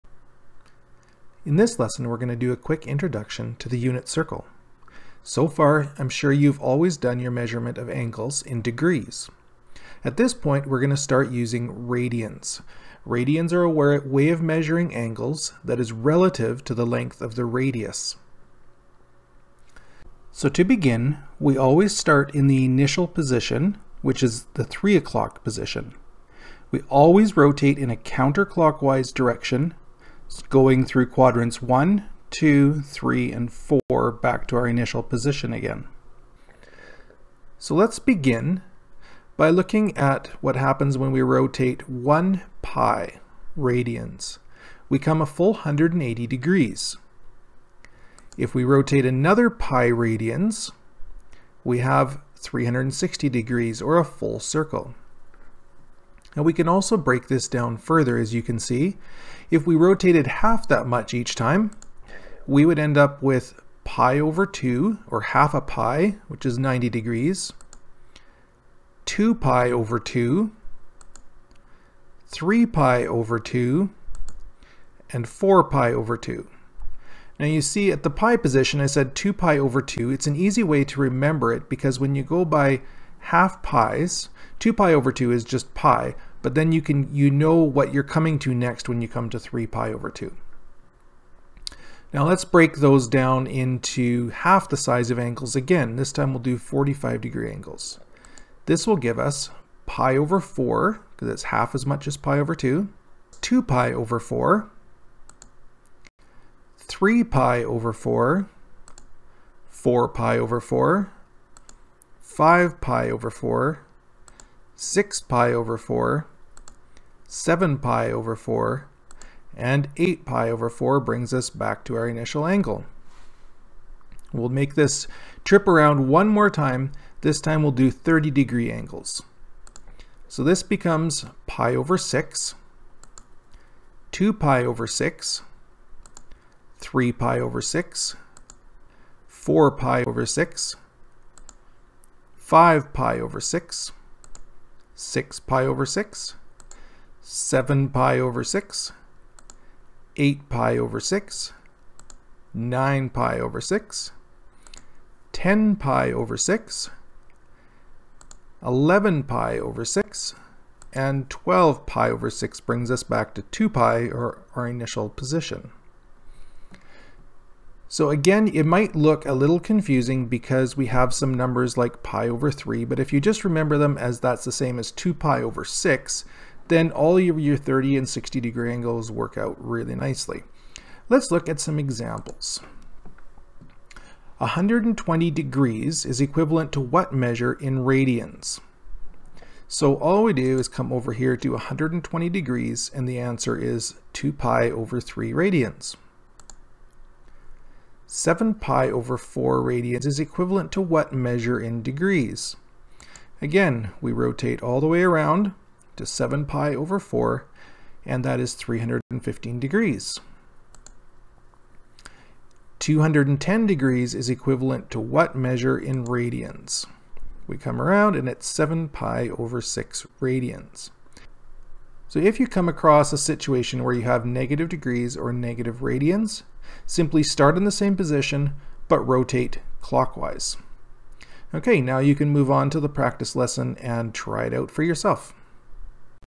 Introduction to Unit Circle
This is a quick introduction to the unit circle and measuring angles in radians